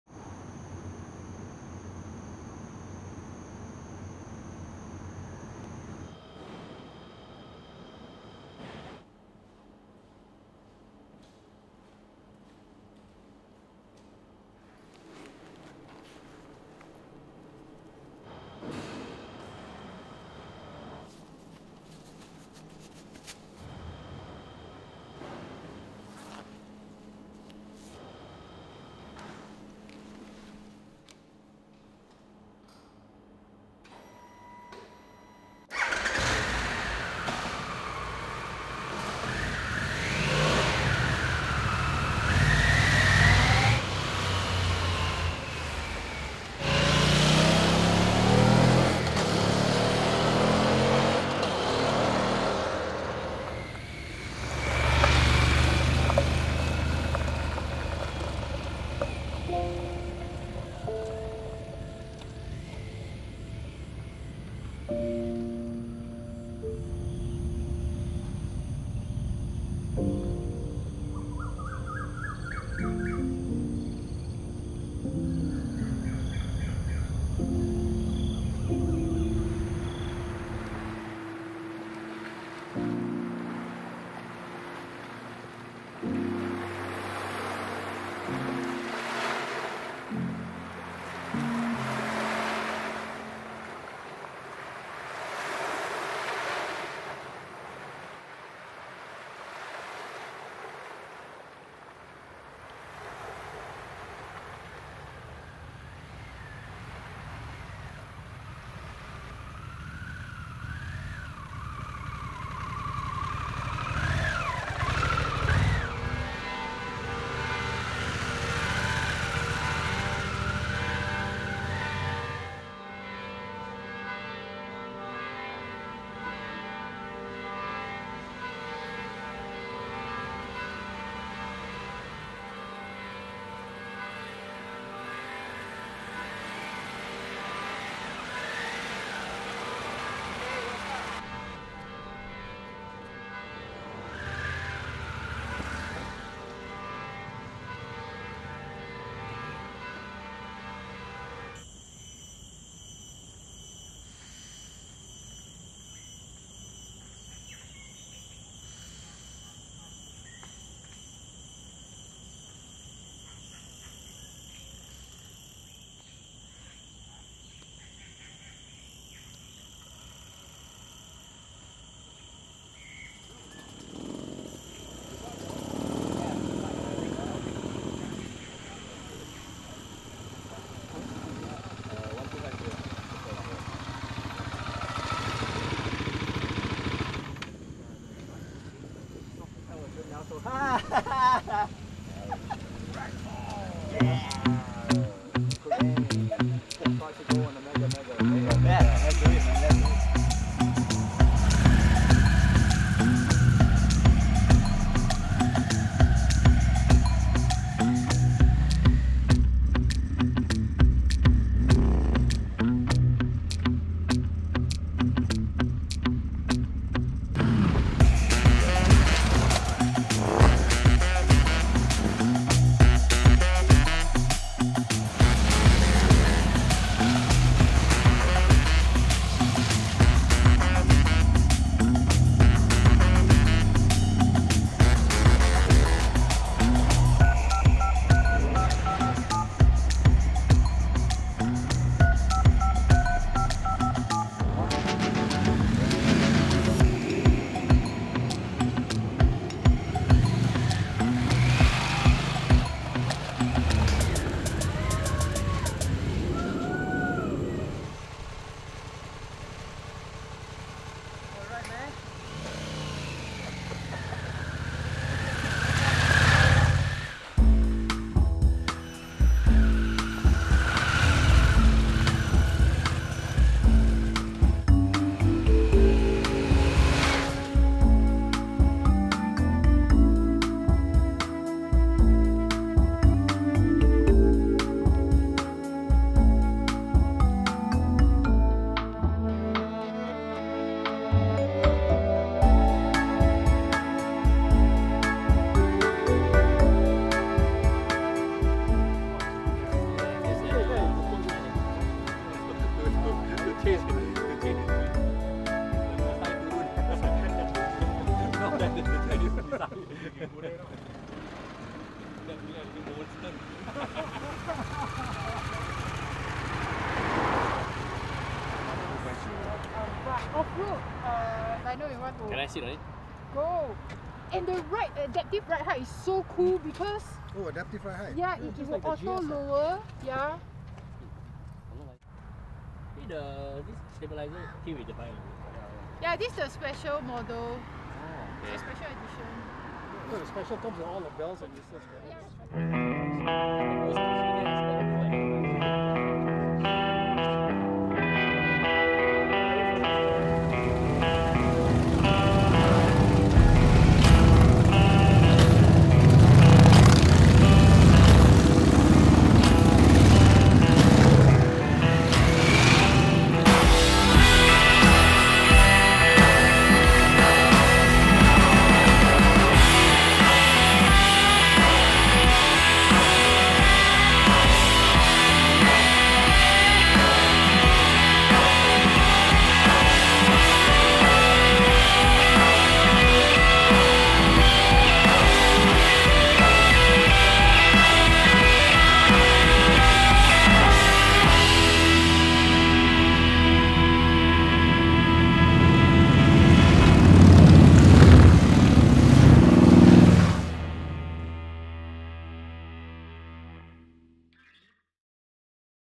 Sunup to Sundown with the Harley-Davidson Pan America
For restless souls marooned in Singapore during an endless pandemic, 2021 has been a fog of unbearably long days. If you had 24 hours with the Harley-Davidson Pan America, on this flat piece of land surrounded by sea, how would you make the most of it?

Sunup to sundown. No monologues, or spec sheets. Just a human, a motorcycle, and their friends. Exactly what modern adventure-motolife should be.

This experience is best enjoyed with headphones. Rock on!

SyncID: MB01OMILGN2PLQW